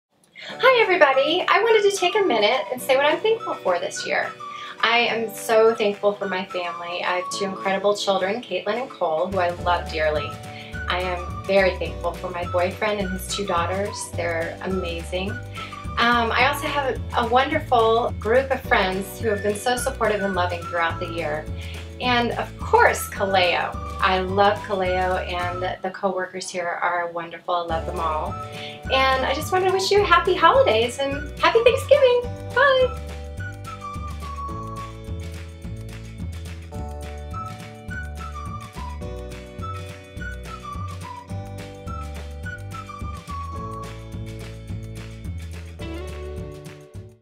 KALEO Thankful - Sarah Jimenez
🍂It's our 17th video of KALEOThankful! 🍂Watch to see what Sarah Jimenez is thankful for this year!